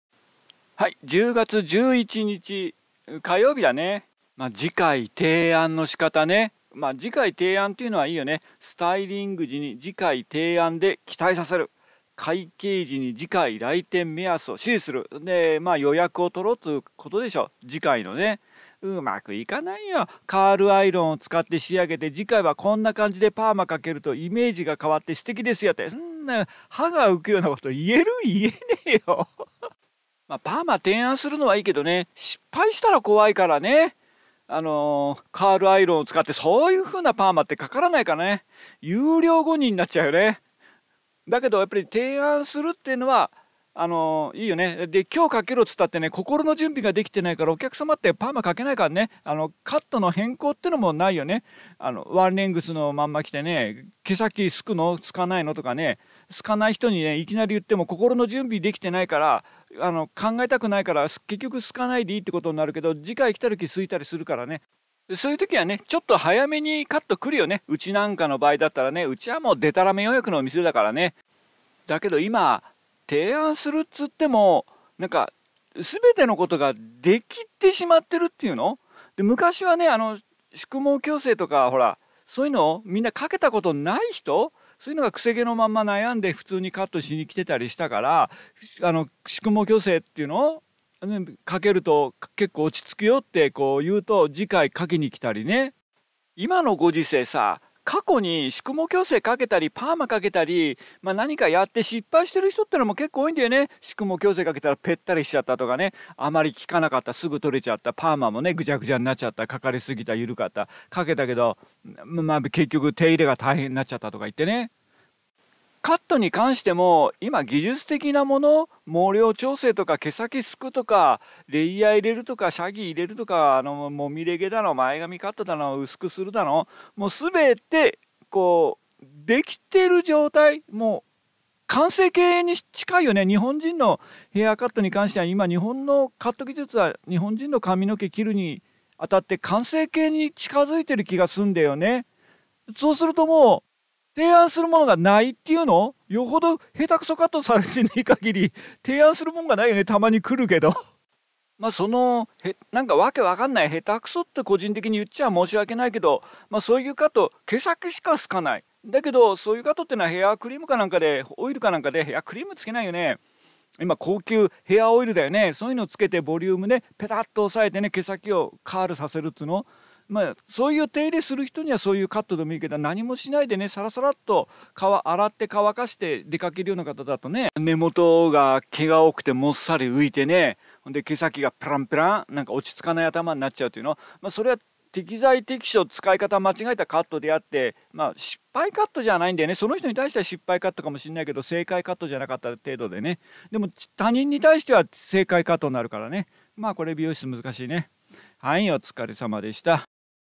おっさん美容師の一人朝礼、美容技術の次回提案と今の日本人カット
今の日本人用カットは完成形に近い、よほど流行が変わらない限り提案する内容がない
質問、疑問、コメント等は都合により過去動画にお願いします